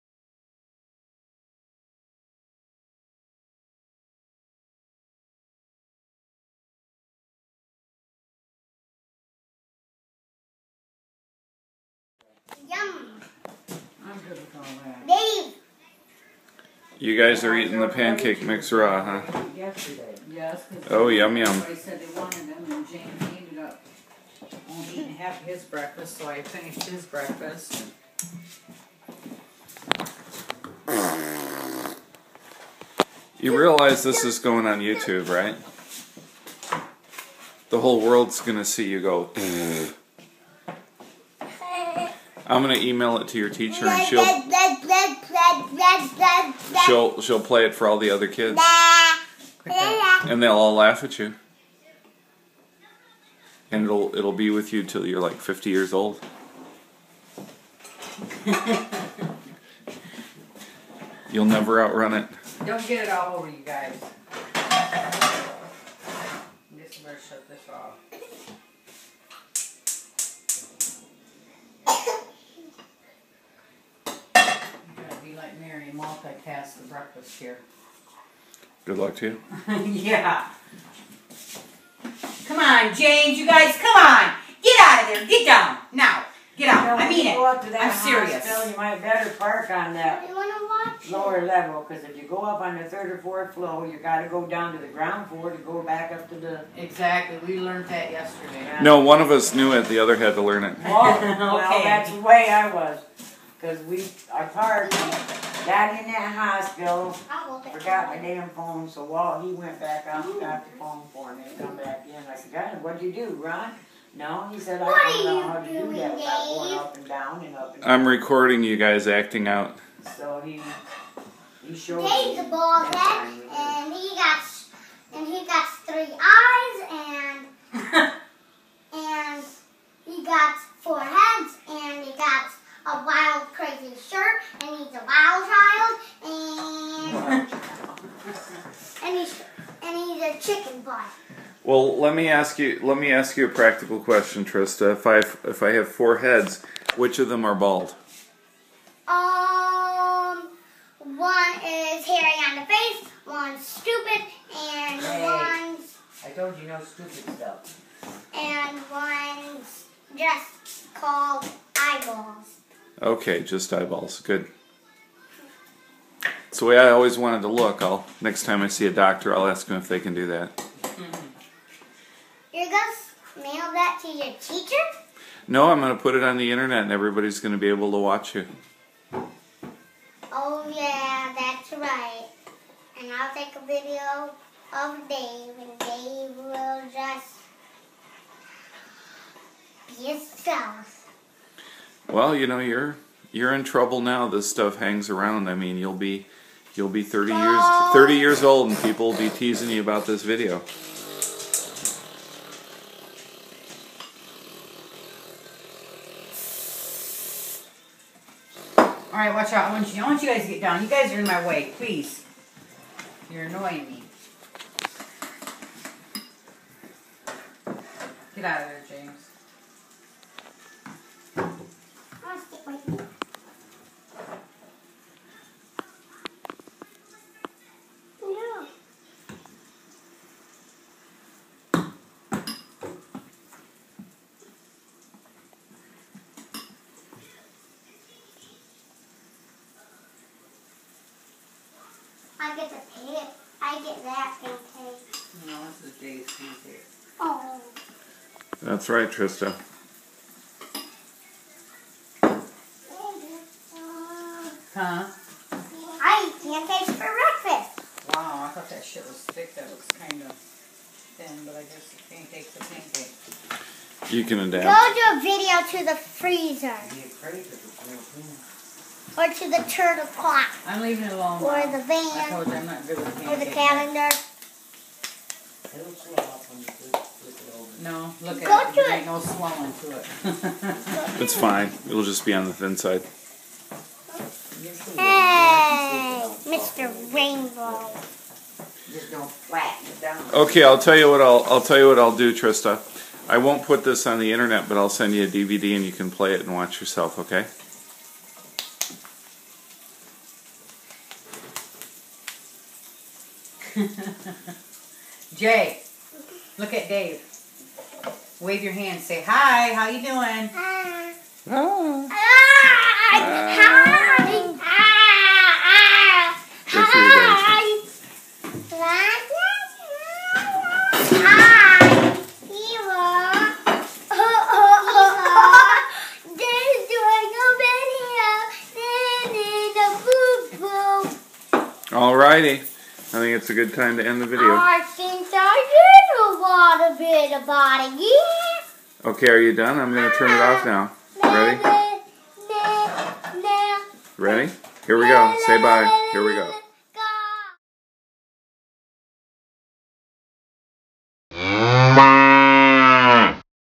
Trista Hamming It Up, July 8, 2012
Trista, James, Dave, Janet, and Mary in Homer, Michigan on July 8, 2012.